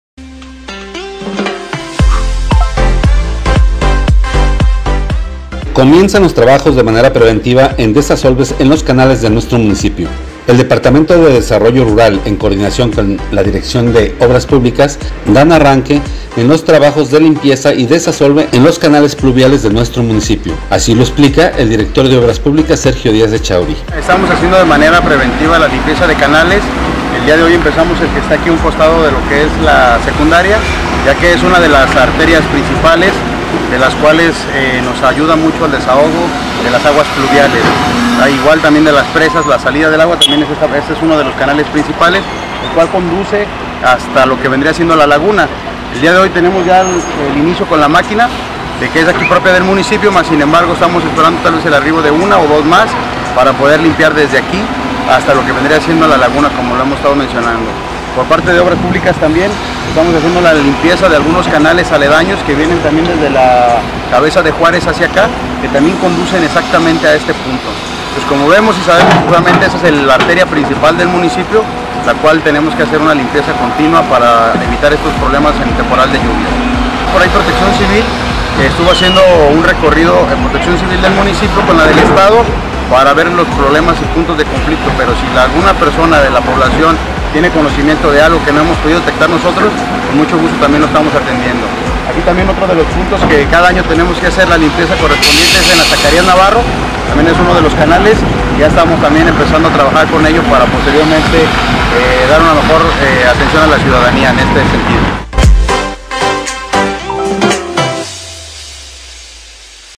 INICIAN LOS TRABAJOS DE DESAZOLVE EN LOS CANALES PLUVIALES DE NUESTRO MUNICIPIO.
La Dirección de Desarrollo Rural de  en coordinación con la Dirección Obras Públicas comienzan los trabajos preventivos de desazolve en los canales pluviales del municipio.